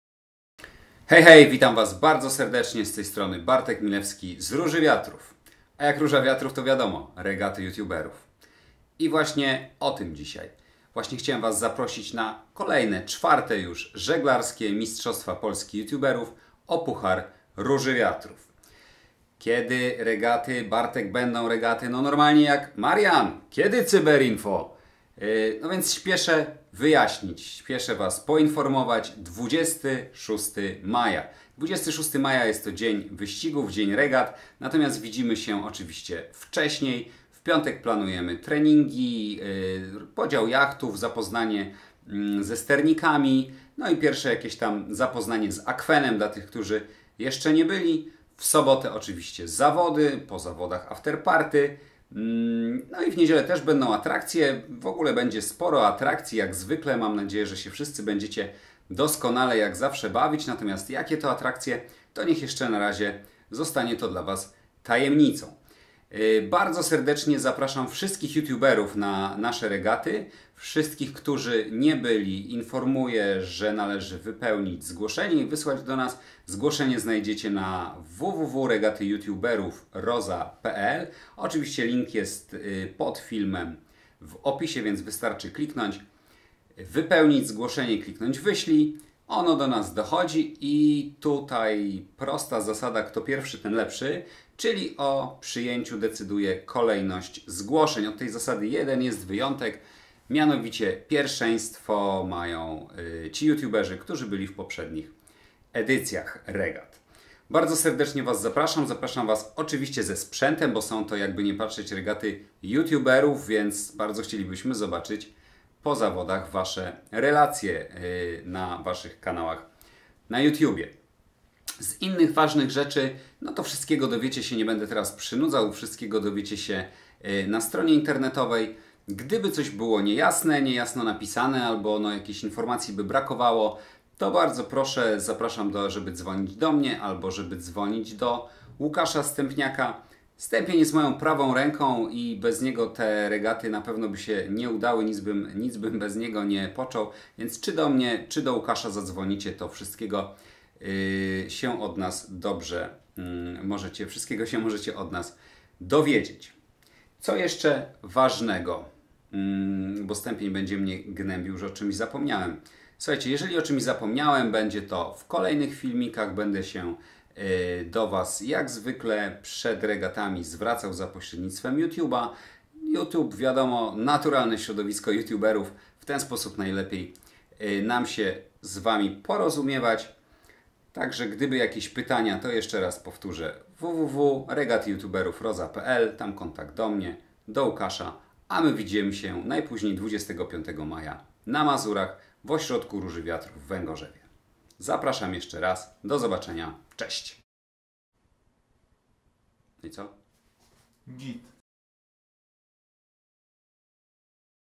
Zaproszenie na 4 Regaty Youtuberów o puchar Róży Wiatrów
Serdecznie zapraszamy wszystkich Youtuberów na 4 Żeglarskie Mistrzostwa Polski Youtuberów, które odbędą się 26 maja 2018 roku w ośrodku Róży Wiatrów na Mazurach!
ZAPRASZAMY